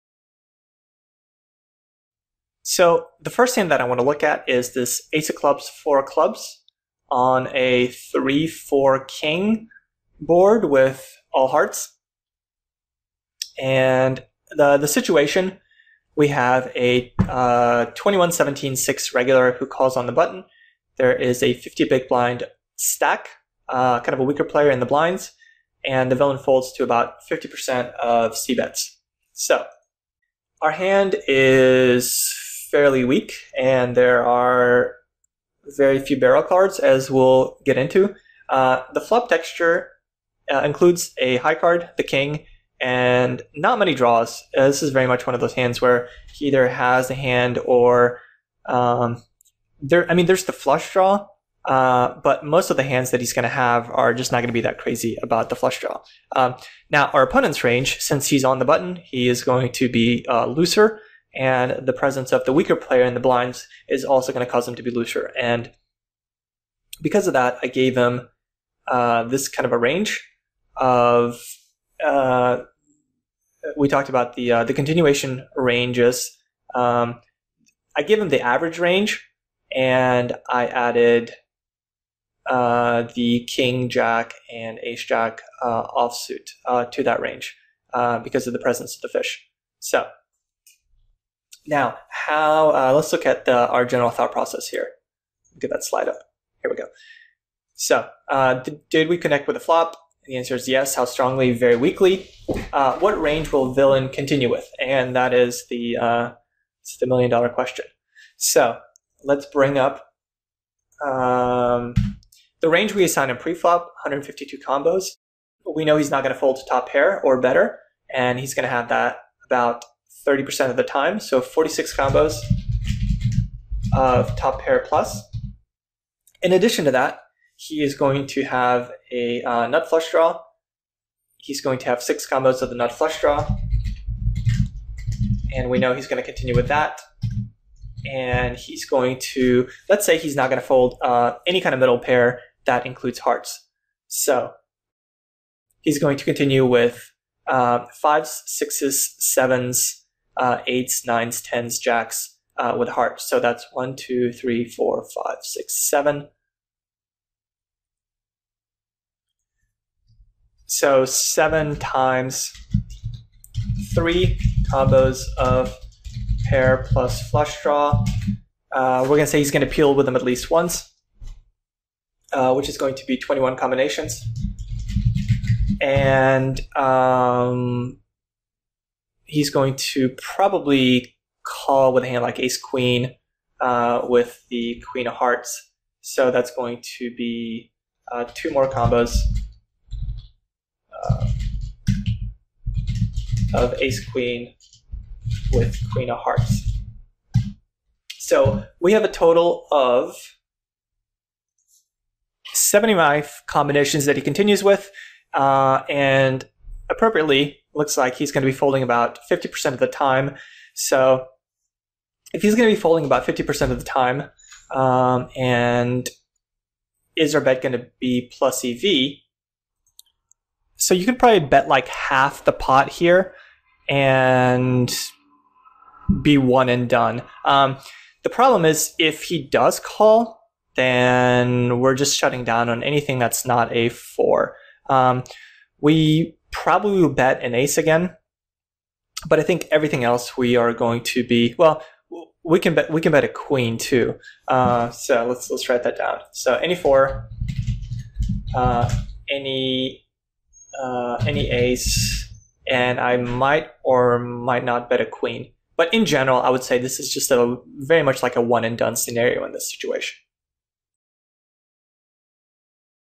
Verneer Crushing The Micros: 2013, Part 3, CardRunners 03.27.13
Crushing The Micros: 2013, Part 3

In this Quickie, Verneer discusses postflop play from early position in a single raised pot. Similar to part two, Pawel discusses the interplay between your hand, the flop texture, and your opponent's range.